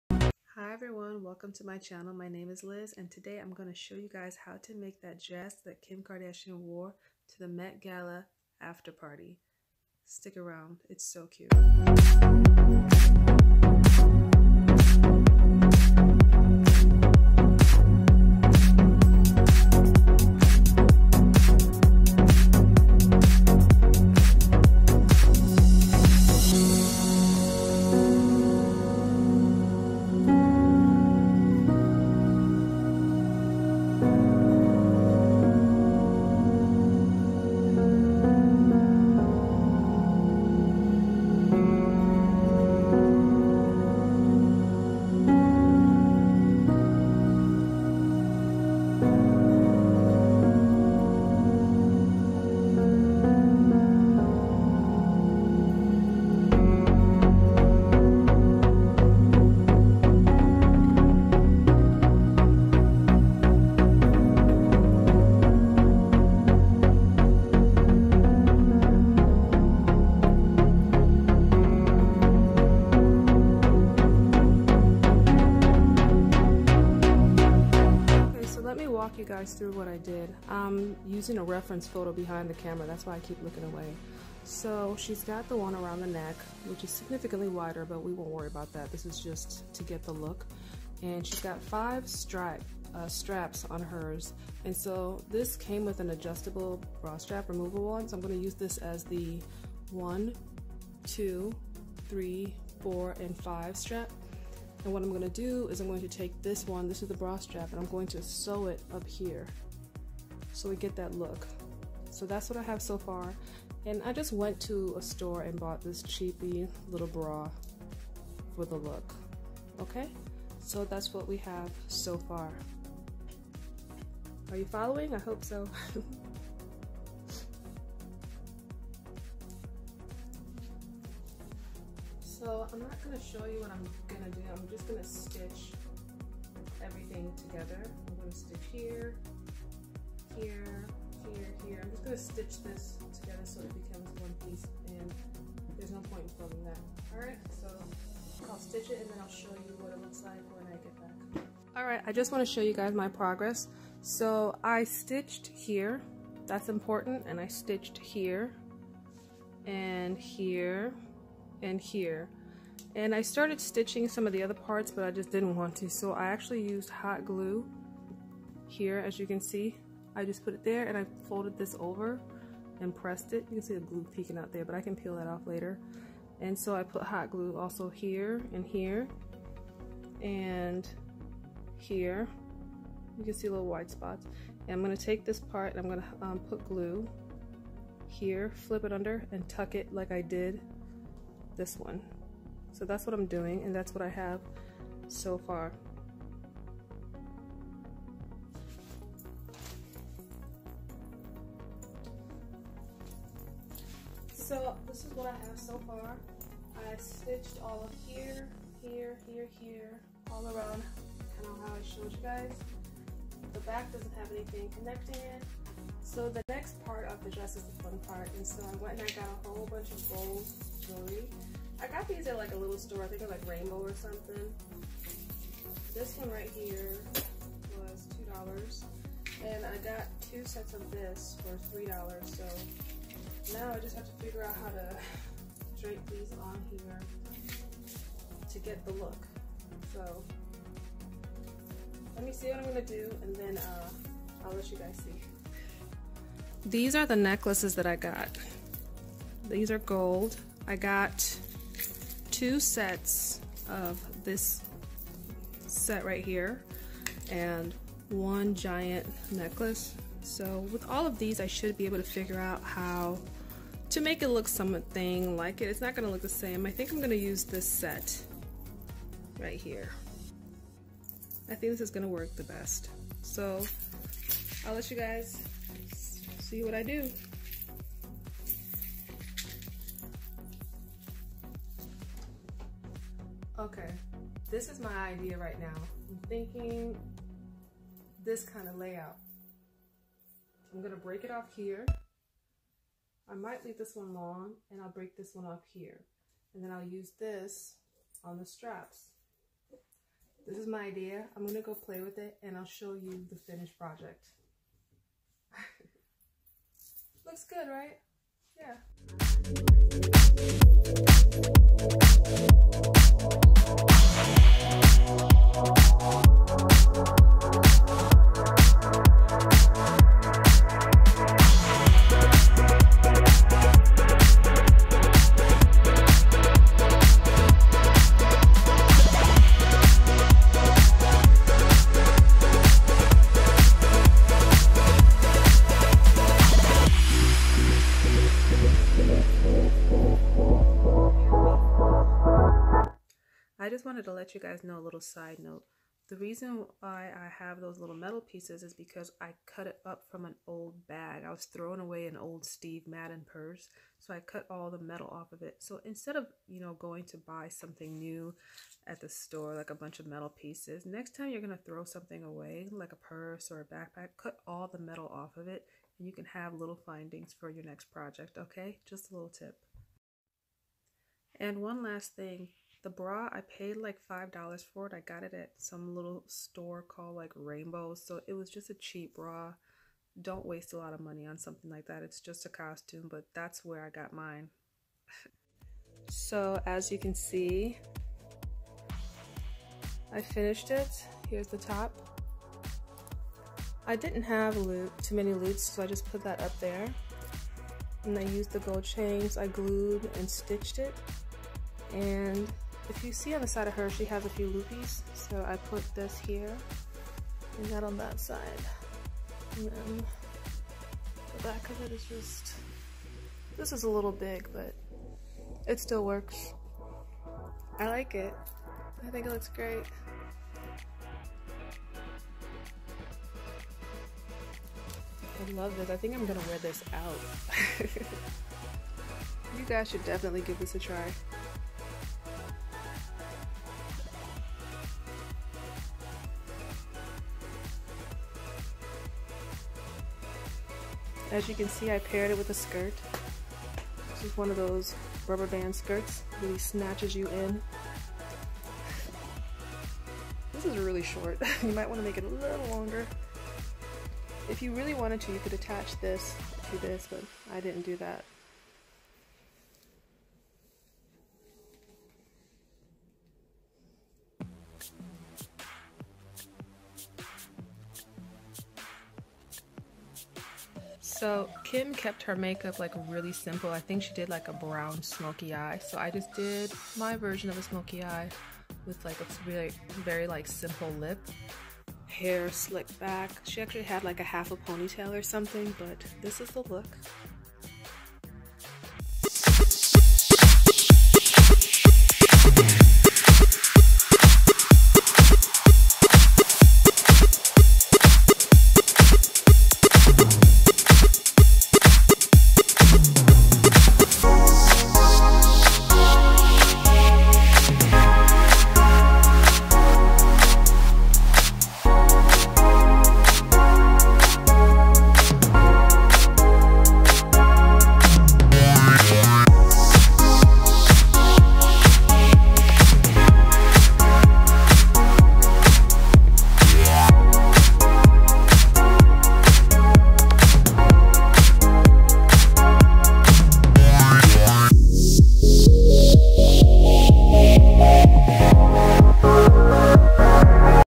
DIY Kim Kardashian Met Gala 2018 After Party Dress
In this video I show you how to make  Kim Kardashian's 2018 met gala Versace dress.